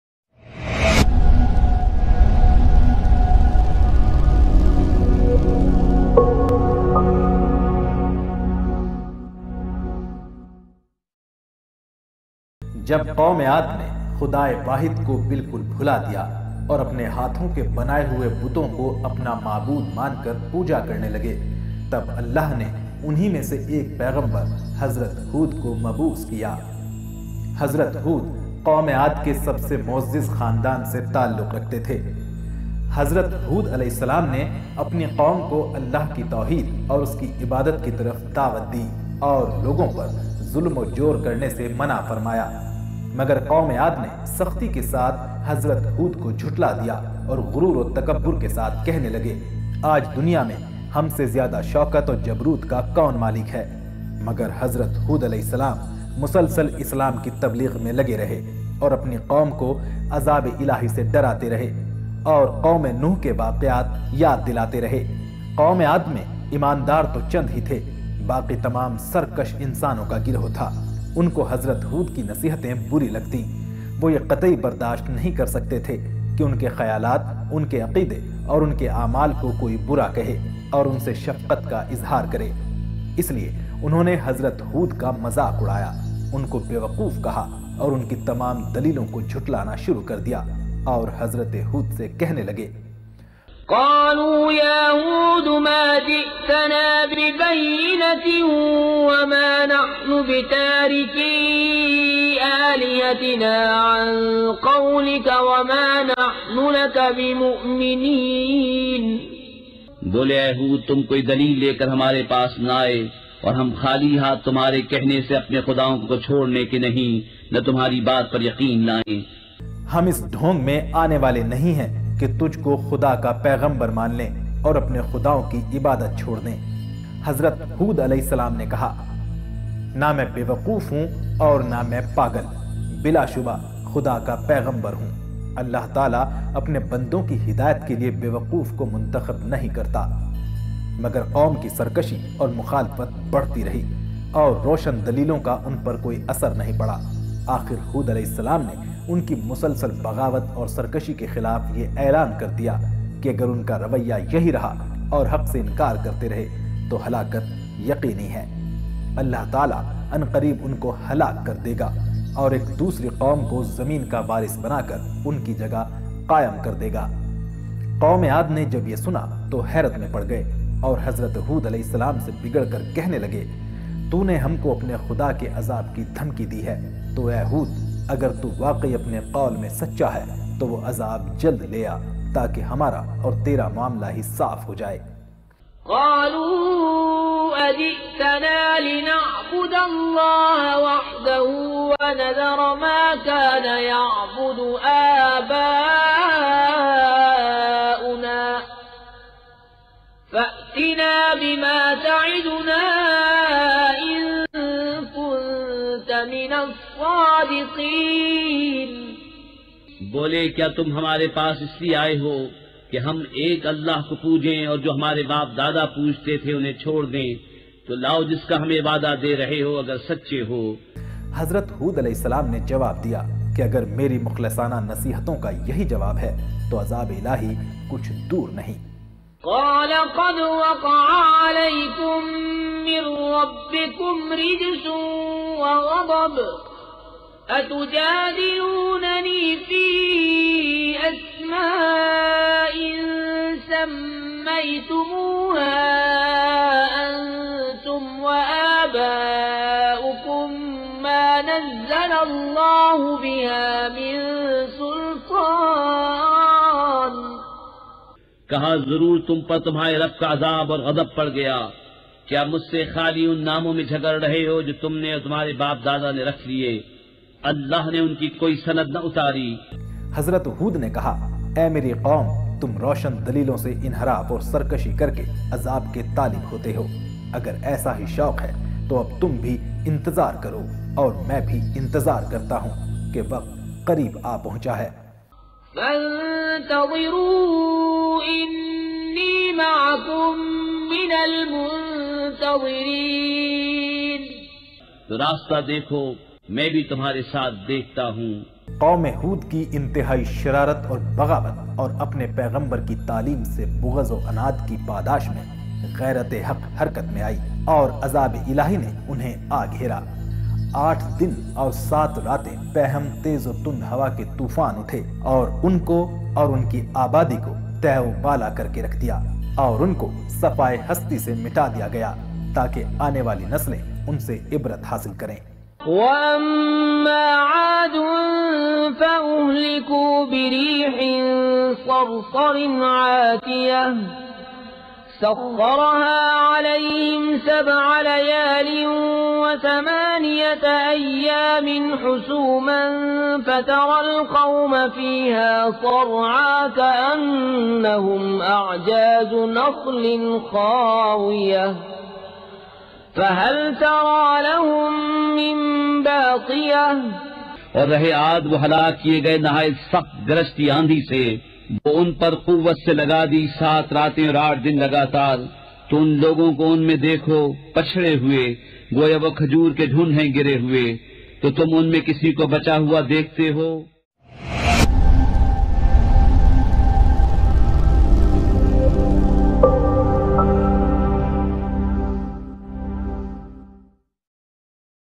जानिए क़ौम ए आद क्यों तबाह हुई
Story of qaumeaad in the view of Qur'an.